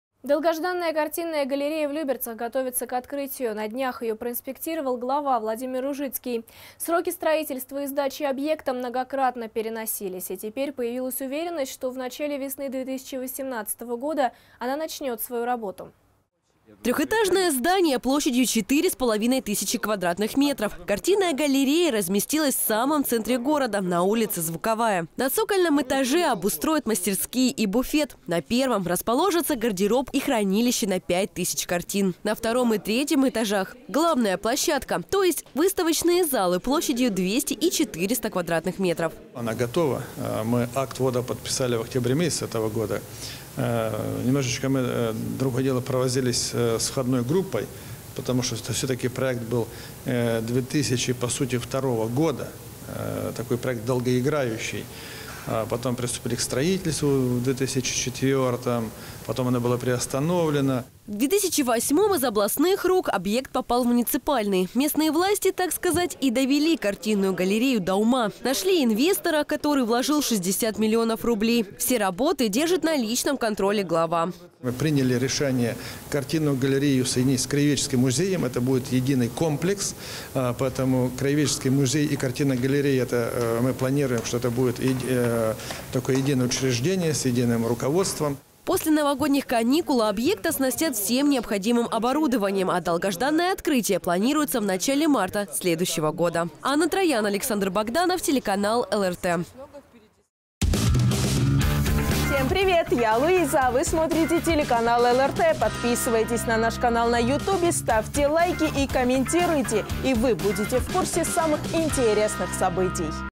Картинная галерея в Люберцах готовится к открытию
Долгожданная картинная галерея в Люберцах готовится к открытию. На днях ее проинспектировал глава Владимир Ружицкий. Сроки строительства и сдачи объекта многократно переносились, и теперь появилась уверенность, что в начале весны 2018-го она начнет свою работу.

Трехэтажное здание площадью 4,5 тысячи квадратных метров. Картинная галерея разместилась в самом центре города, на улице Звуковая. На цокольном этаже обустроят мастерские и буфет. На первом расположатся гардероб и хранилище на пять тысяч картин. На втором и третьем этажах – главная площадка, то есть выставочные залы площадью двести и четыреста кв.м..

Владимир Ружицкий, глава городского округа Люберцы:
"Она готова. Мы акт ввода подписали в октябре месяце этого года. Немного провозились с входной группой, потому что проект был по сути 2002 года. Проект долгоиграющий. Приступили к строительству в 2004, потом оно было приостановлено..."

В 2008-м из областных рук объект попал в муниципальные. Местные власти, так сказать, и довели картинную галерею до ума. Нашли инвестора, который вложил 60 млн.рублей, все работы держит на личном контроле главы.

"Мы приняли решение, картинную галерею соединить с краеведческим музеем, это будет единый комплекс. Это будет единое учреждение с единым руководством.."

После новогодних каникул объект оснастят всем необходимым оборудованием. А долгожданное открытие планируется в начале марта следующего года.

Анна Троян, Александр Богданов